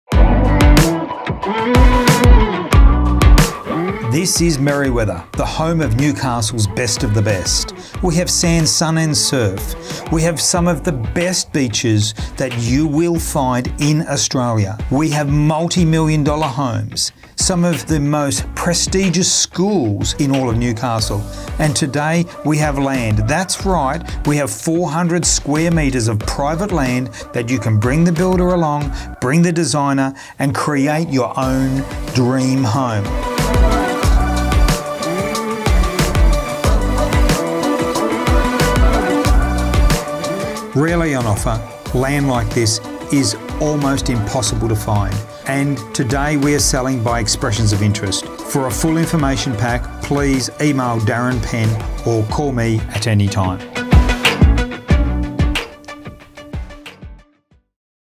65 Yule Road Merewether
Merewether is home to some of the best beaches and homes in Newcastle, and up for grabs is 400 square meters of land only minutes from beaches, schools and shopping.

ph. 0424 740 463